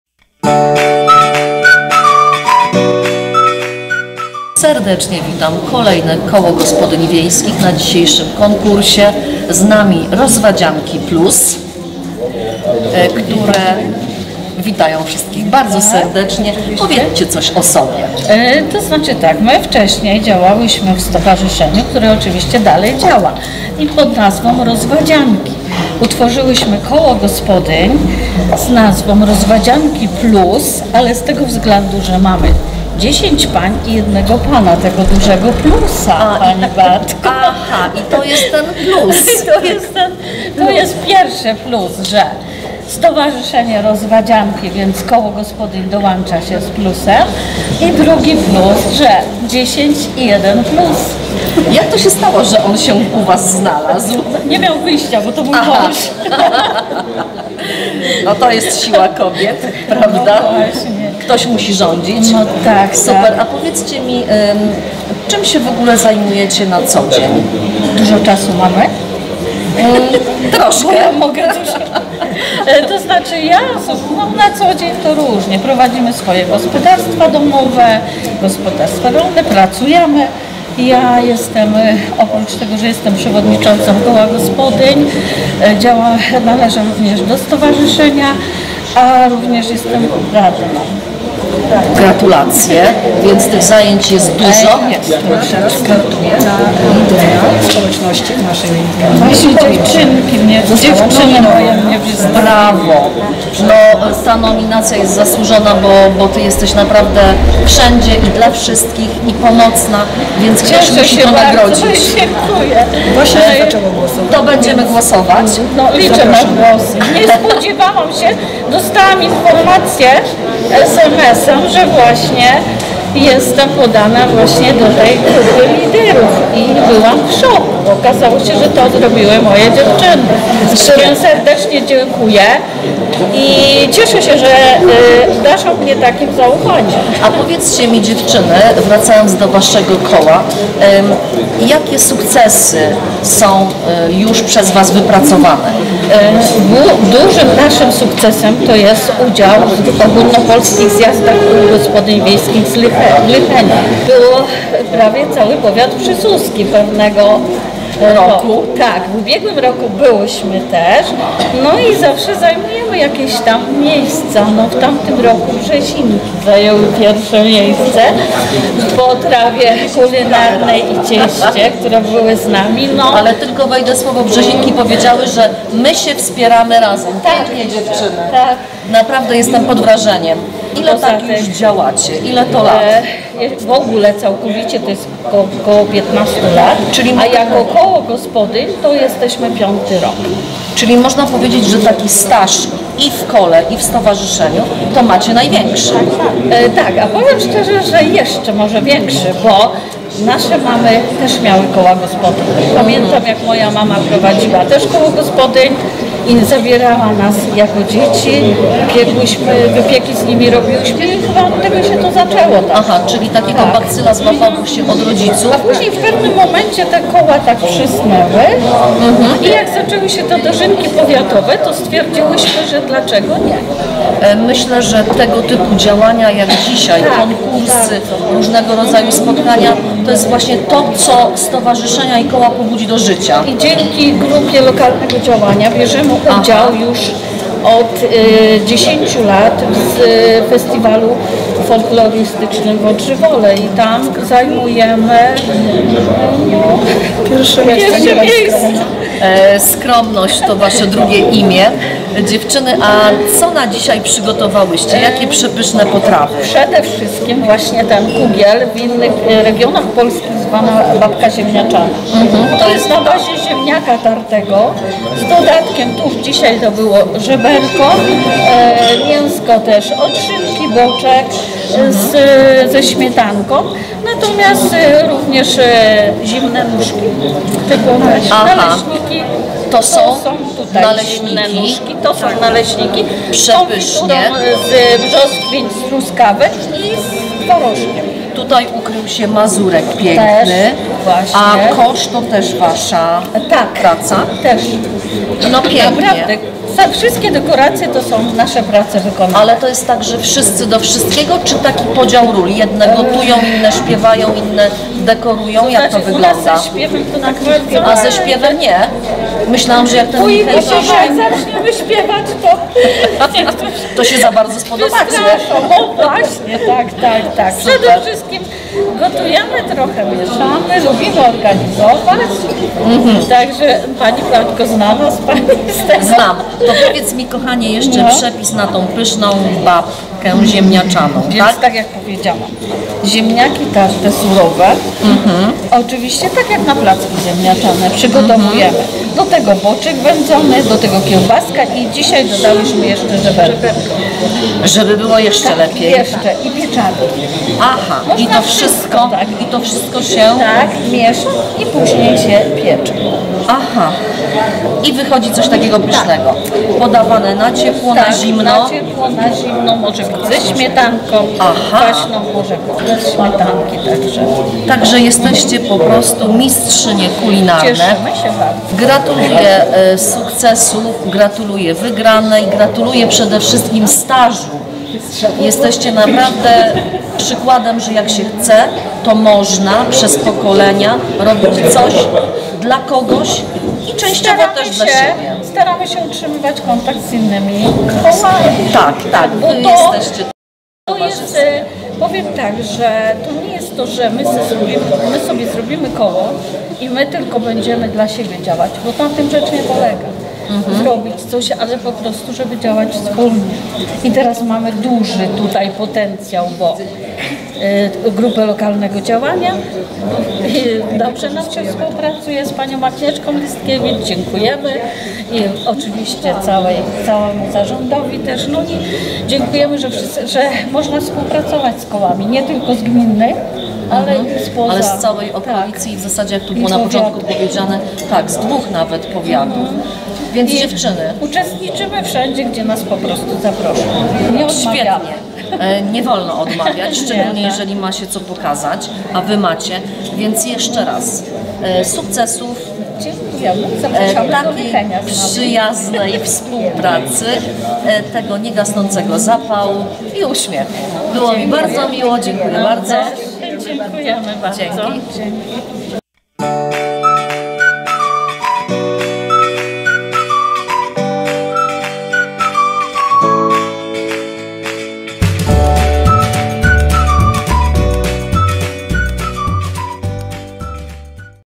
odc. 5 - KGW Rozwadzianki Plus z Rozwad
Tym razem przedstawiamy KGW Rozwadzianki Plus z Rozwad i ich główną potrawę konkursową.Posłuchajcie i zobaczcie co robią dziewczyny i jak im to wychodzi.